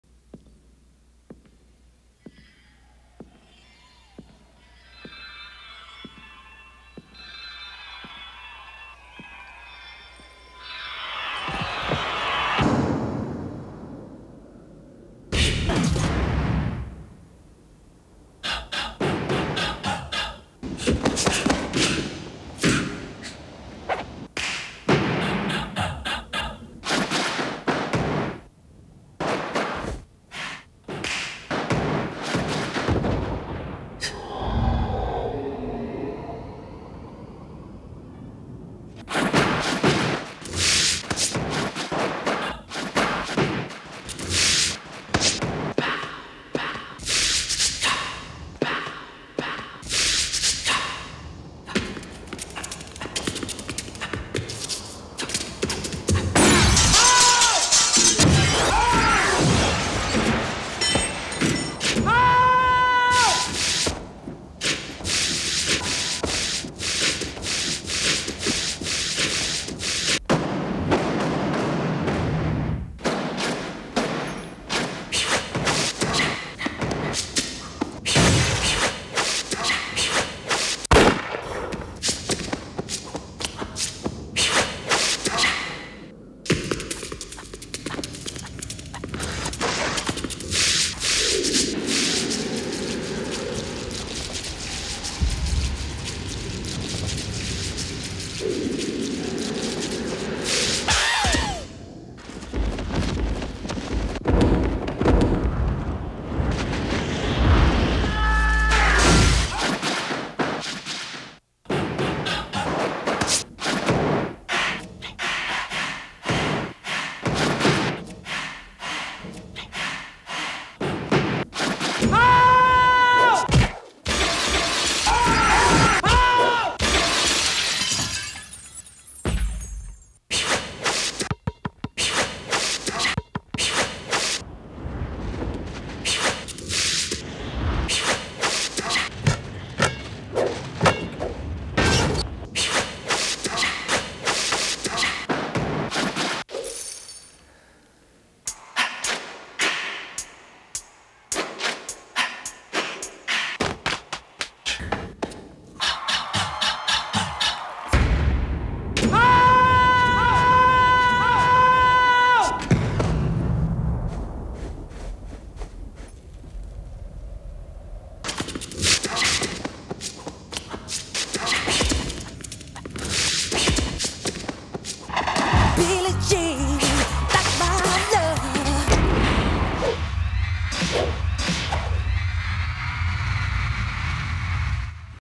MY TRIBUTE TO MICHAEL JACKSON- BEST DANCE MONTAGE***
My tribute to the legend mIchael Jackson, featuring all his best dance moves rolled into one piece...enjoy